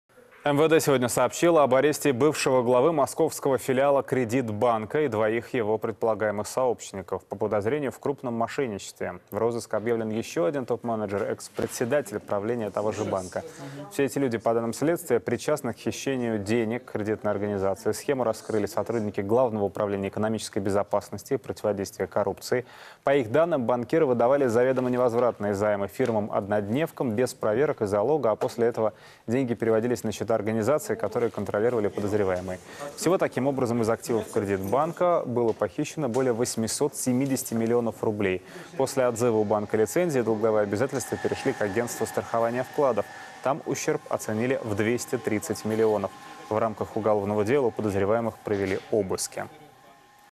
Бывший руководитель московского филиала Кредит-Банка арестован по подозрению в мошенничестве
МВД сегодня сообщило об аресте бывшего главы московского филиала Кредит-Банка и двоих его сообщников по подозрению в крупном мошенничестве. В розыск объявлен ещё один топ-менеджер - экс-председатель правления того же банка.